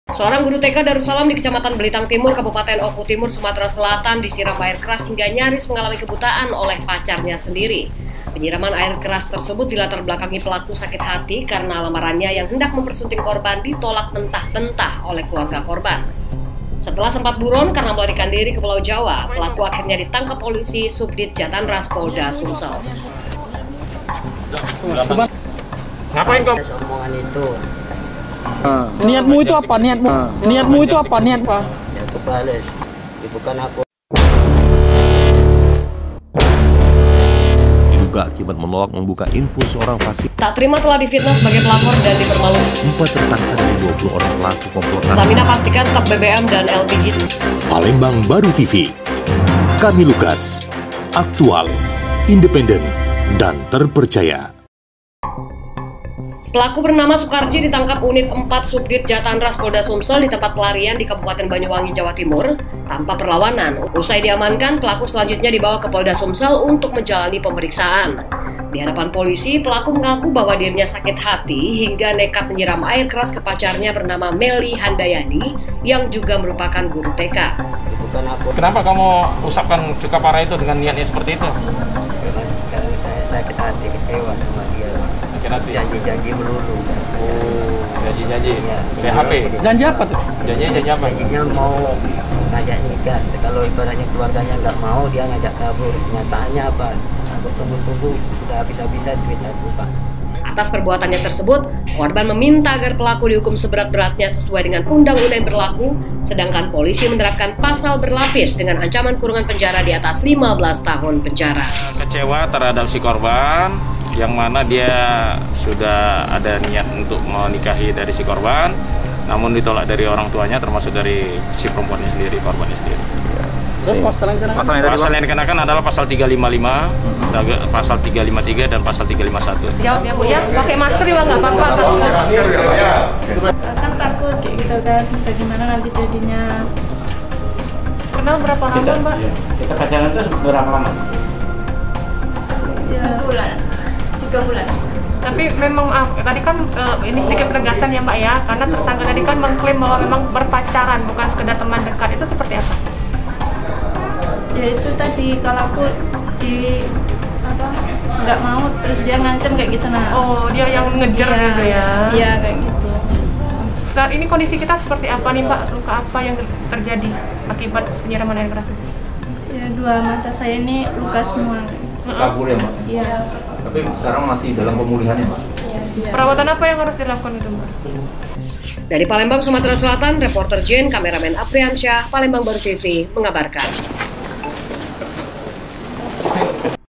Tolak Ajakan Nikah Muka Guru TK di Usap Air Keras, Pelaku Akhirnya Ditangkap
SHOOTLIST

VISUAL

- PELAKU DI TANGKAP PETUGAS DI BANYU WANGI

- BARANG BUKTI

- KORBAN

- TERSANGKA

- SOT // SUKARJI / TERSANGKA

- SOT // KOMPOL. CRISTHOPER PANDJAITAN / KASUBDIT JATANRAS POLDA SUMSEL //

- SOT // MELI HANDAYANI / KORBAN GURU TK DARUSALAM OKU TIMUR

ARI PALEMBANG SUMATERA SELATAN// REPORTER / JANE // KAMERAMEN  / APRIYANSA PALEMBANGBARU MENGABARKAN